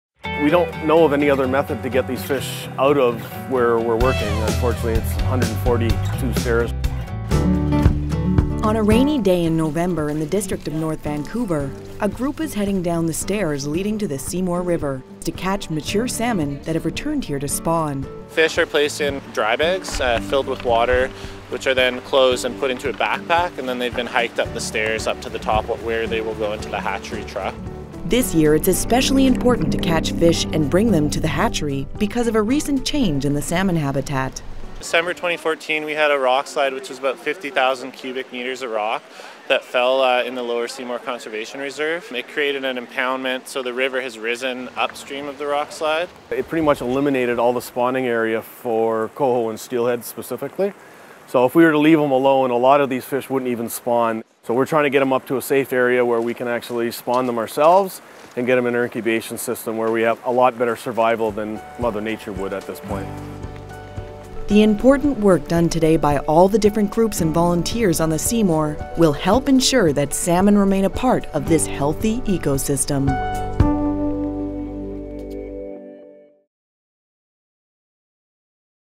Seymour River Fish Trapping Metro Update
Migrating salmon up the Seymour River get human help to make it past a massive rock slide that came down in the area in December 2014. Fish are trapped, carried in backpacks up 140+ stairs and then trucked to a hatchery to continue their lifecycle.